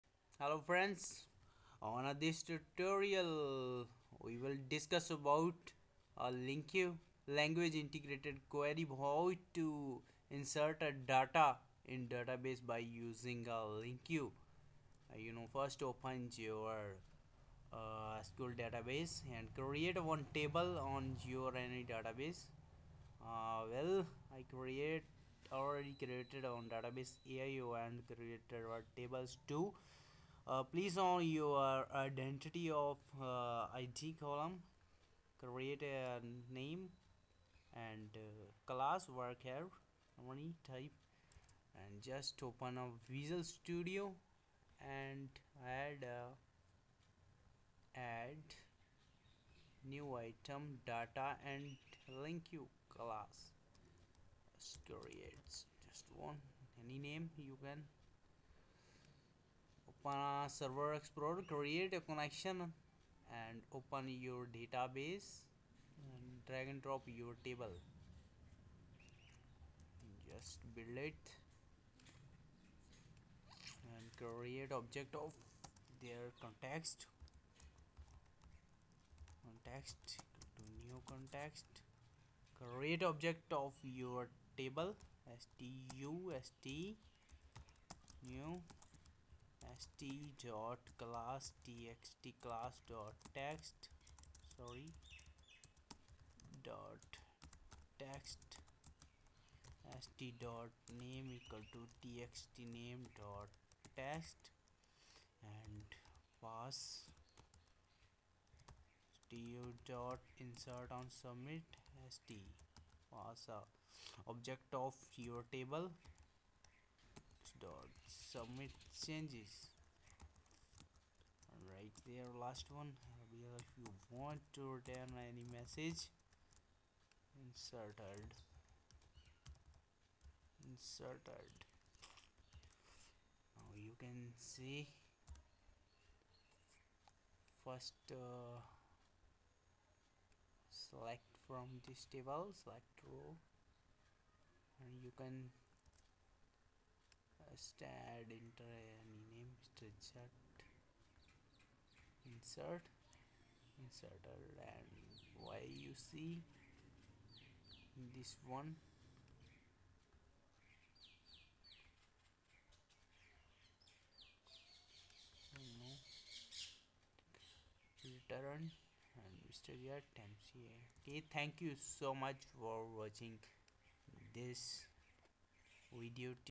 Insert Data By Using Linq On Database asp net
on this tutorial we can discus about a how to insert data in database by using linq dbml class , and linq technology. also explain all to create DBML file , create database and table by using c#.